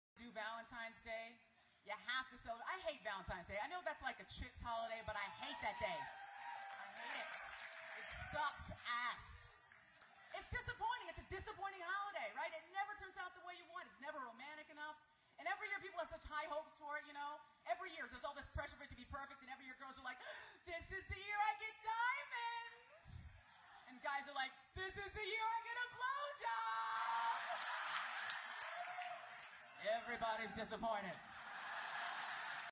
Aisha on valentines day
Aisha Tyler talks about valentines day